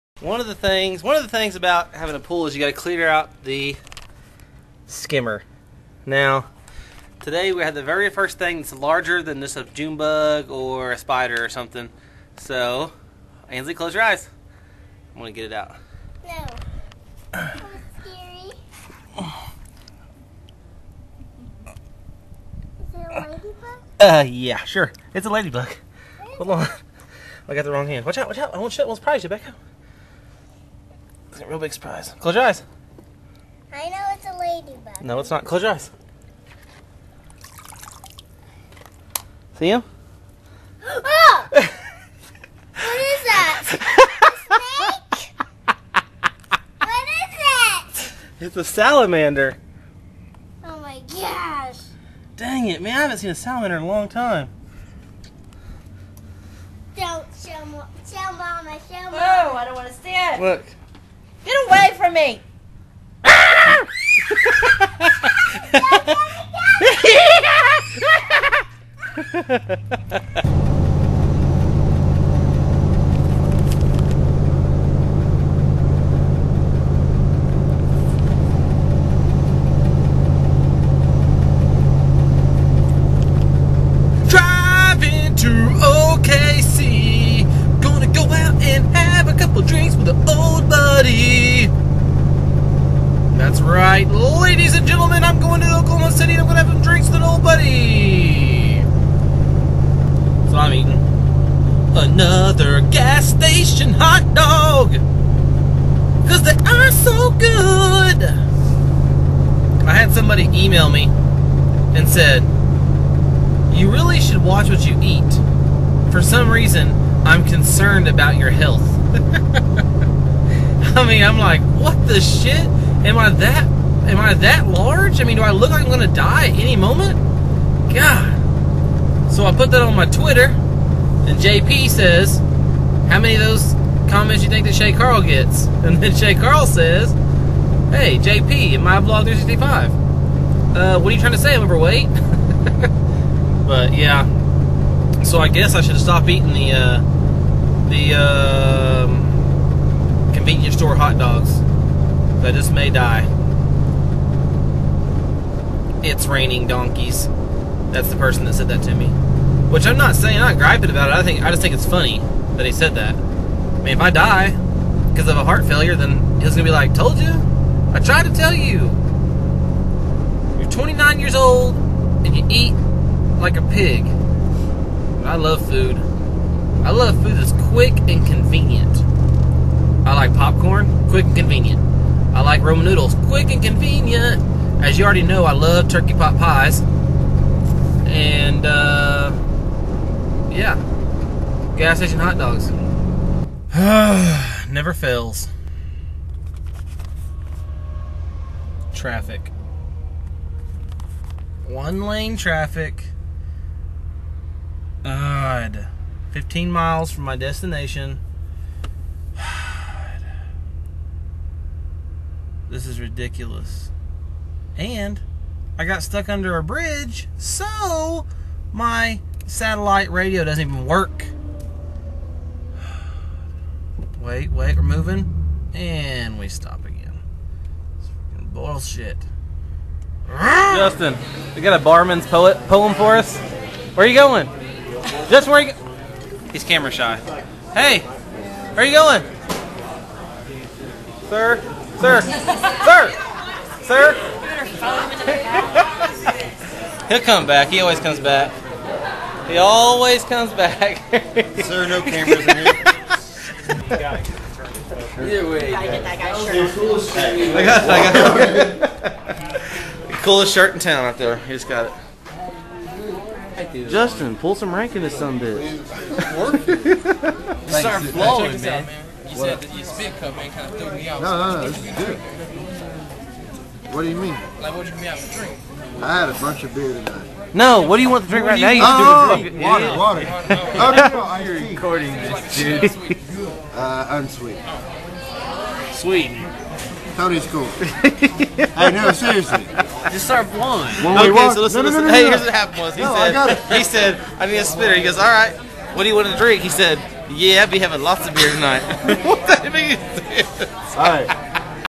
salamander attacks!!! (6.12.09 day 53)
cleaned out the pool and went to visit some friends